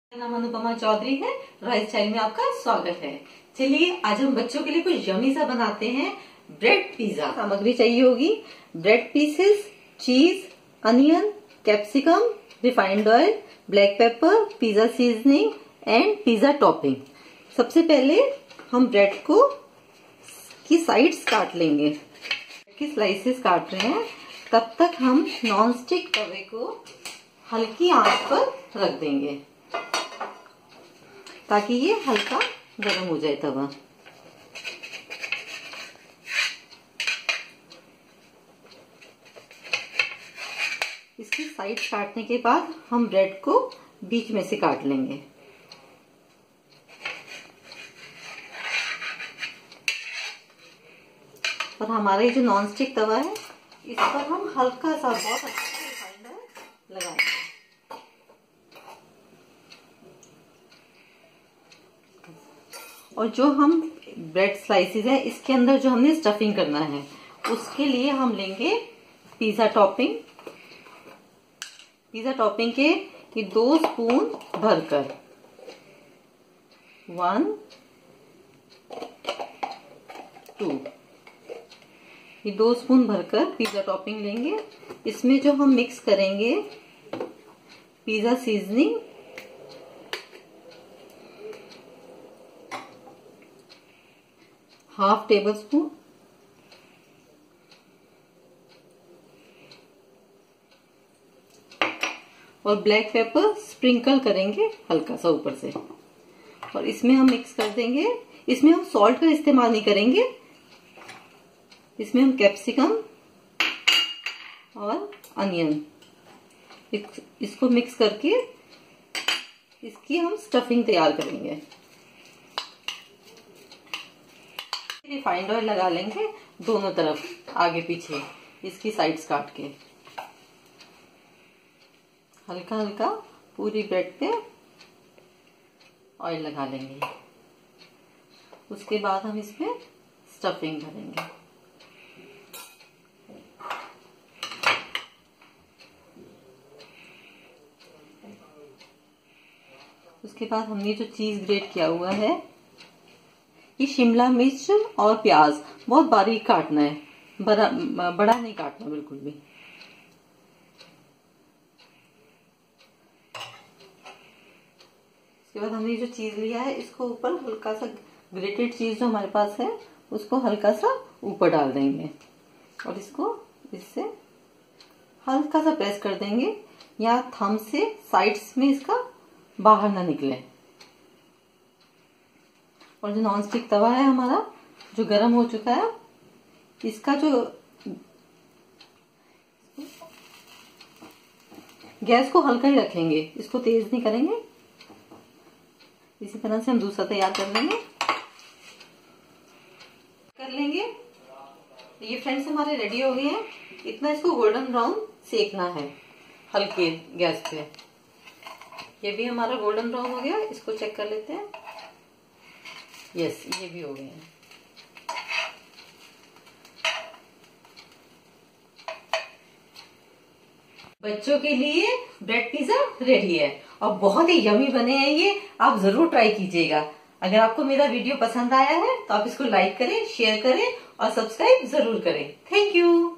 Yummy Bread Pizza
Ingredients :-
Bread Slice
Pizza Topping
Black Pepper
Pizza Seasoning (Oregano)
Cheese
Capsicum - 1
Onion - 1
Refined oil